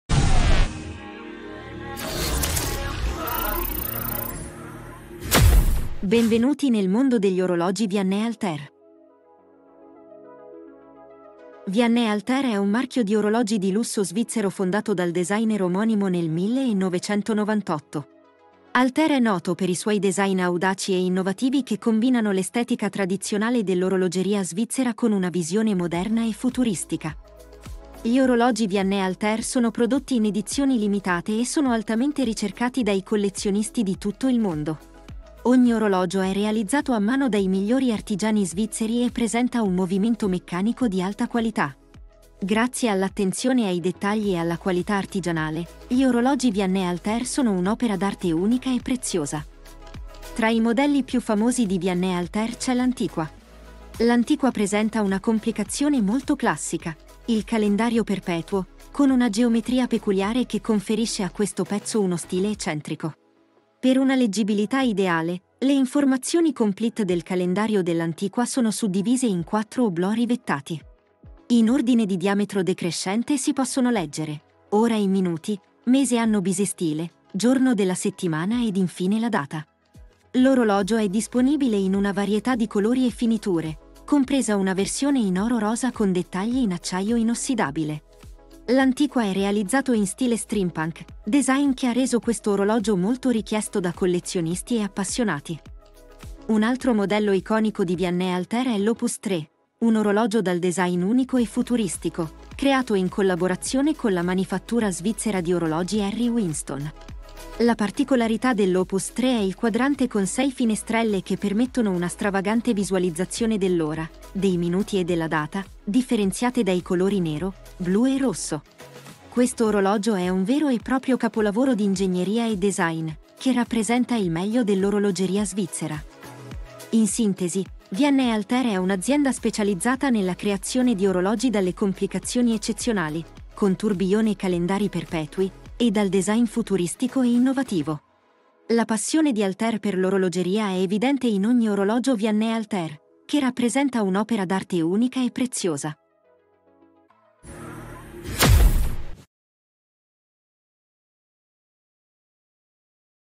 Gli orologi Vianney Halter: artigianato svizzero e design innovativo
Gli orologi Vianney Halter sono il risultato della combinazione tra l'artigianato svizzero e il design innovativo. Ogni orologio è realizzato a mano dai maestri orologiai di Vianney Halter, che utilizzano tecniche tradizionali per creare pezzi unici e dallo stile inconfondibile. In questo video scoprirai gli orologi Vianney Halter, le loro caratteristiche principali e i materiali utilizzati per la loro produzione. Grazie alla loro attenzione ai dettagli e alla loro maestria artigianale, gli orologi Vianney Halter sono dei veri e propri capolavori dell'orologeria. Gli orologi Vianney Halter sono realizzati con materiali di alta qualità, come l'oro e il titanio, che li rendono eleganti e resistenti allo stesso tempo. Inoltre, grazie al loro design innovativo e alla loro originalità, gli orologi Vianney Halter sono adatti a chi cerca un orologio unico e fuori dagli schemi. Scopri gli orologi Vianney Halter in questo video e lasciati conquistare dalla loro bellezza e dalla loro originalità.